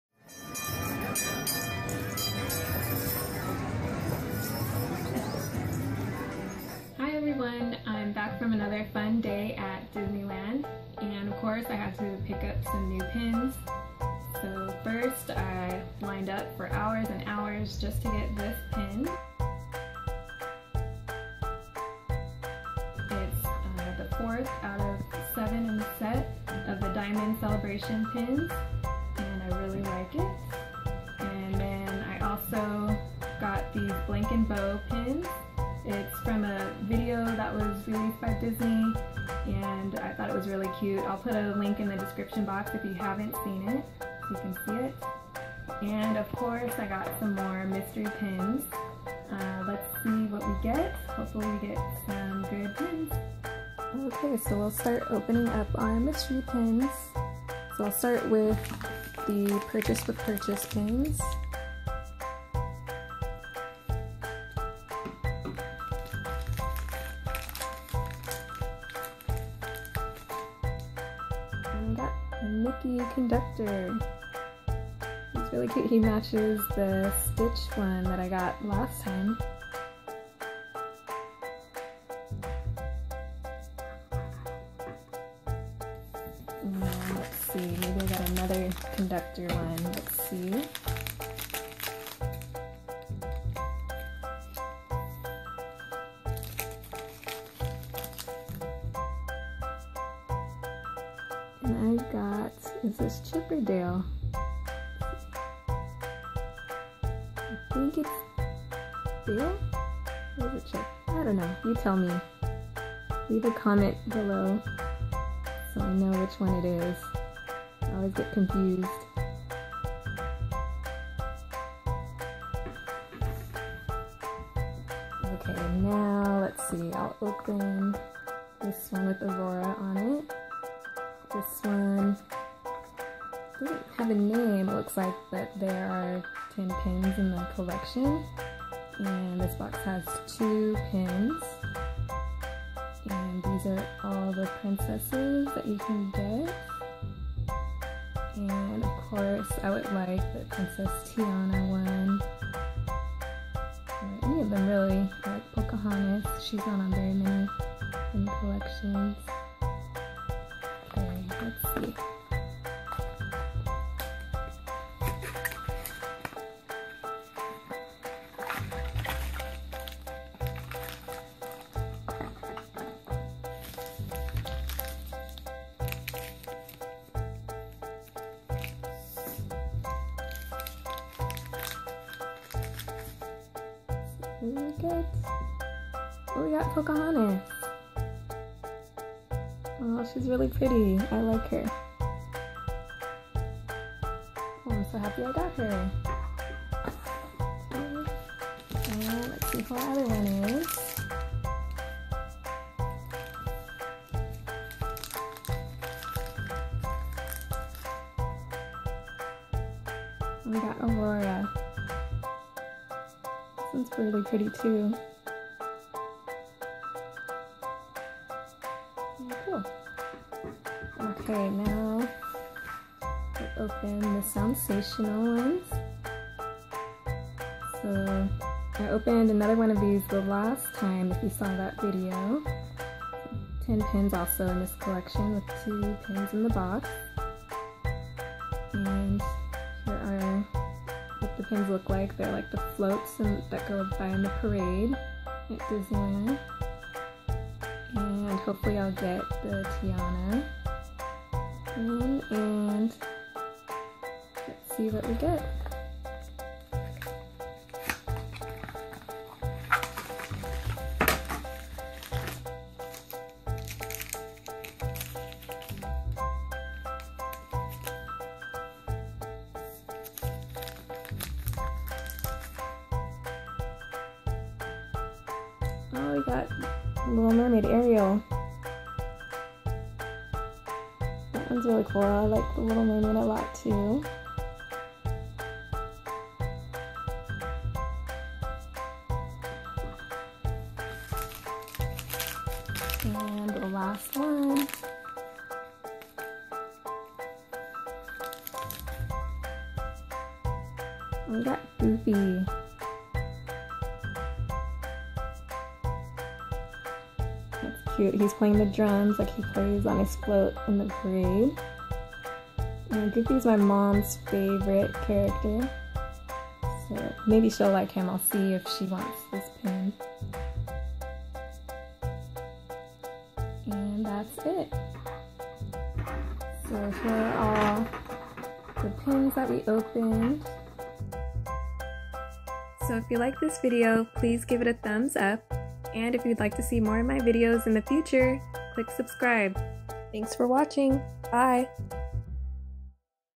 Disney Pin Haul and Mystery Pin Opening 2
Today I'm showing you my pin haul and opening a few mystery pins that I got from Disneyland.
Disneyland 60th Anniversary LE 3000 pin
Blank and Bow pins
Princess Box
Mickey's Soundsational Parade Box
Purchase with Purchase Pins